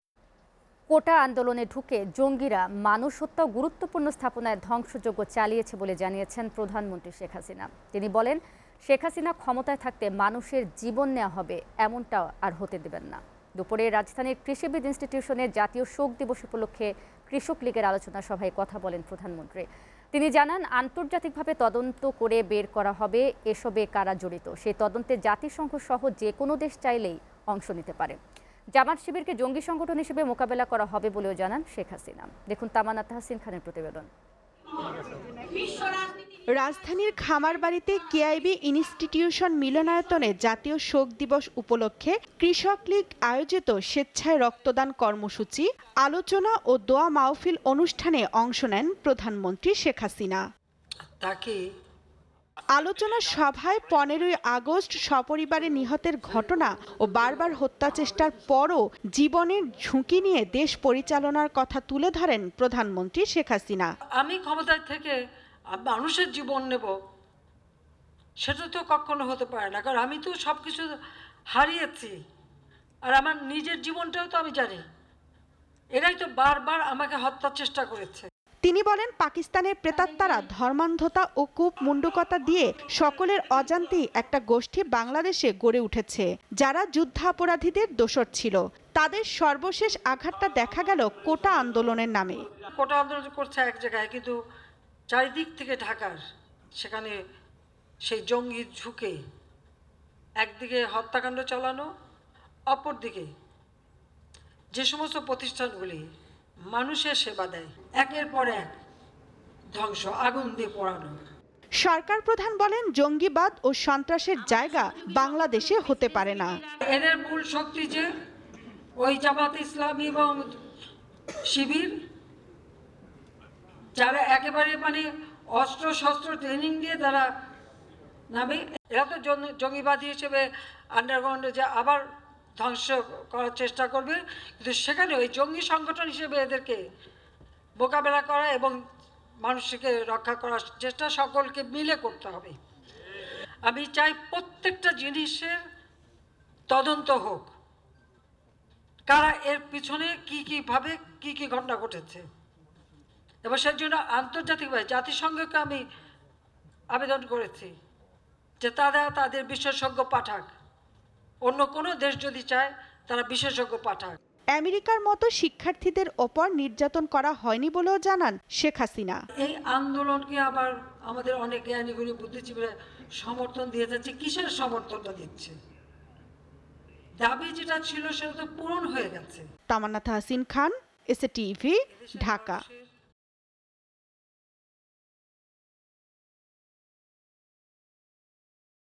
আন্তর্জাতিকভাবে তদন্ত করে বের করা হবে এসবে কারা জড়িত : প্রধানমন্ত্রী | PM | SATV NEWS
আন্তর্জাতিকভাবে তদন্ত করে বের করা হবে এসবে কারা জড়িত : প্রধানমন্ত্রী

SATV Location:
Bangladesh